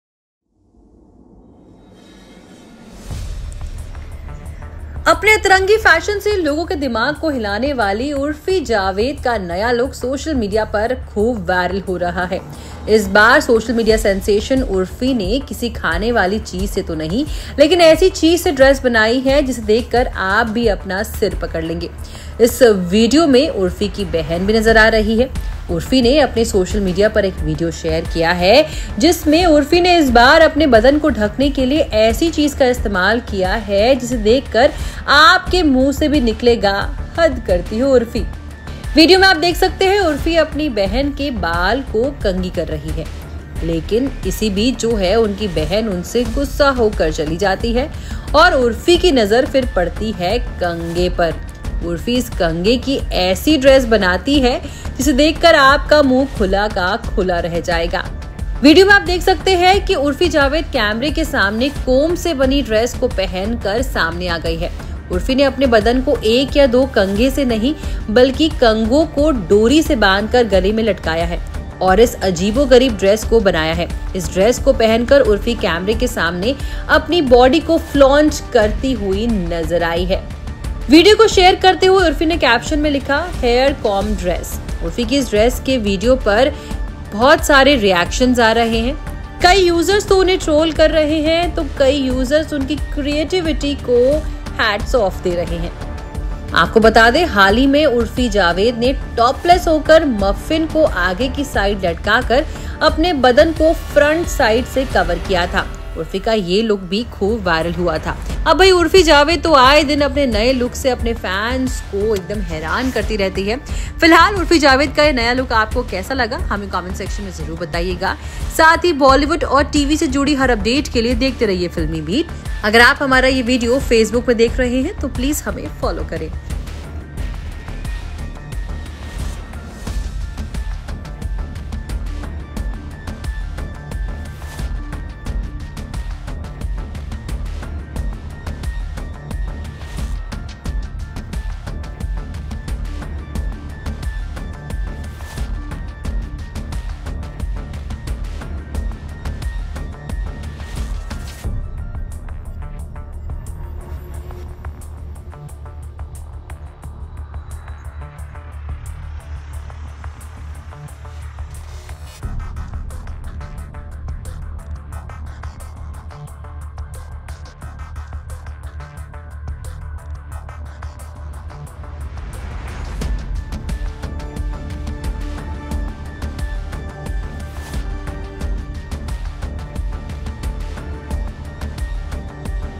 Urfi Javed ने Comb से बनाई अपनी New Dress, Fans को नहीं आई पसंद । FilmiBeat
Urfi Javed is back at grabbing headlines, and this time again she wears an outfit made of a Comb. This time fashionista Urfi Javed made fans wait a little too long for her new look. Watch Video to know more...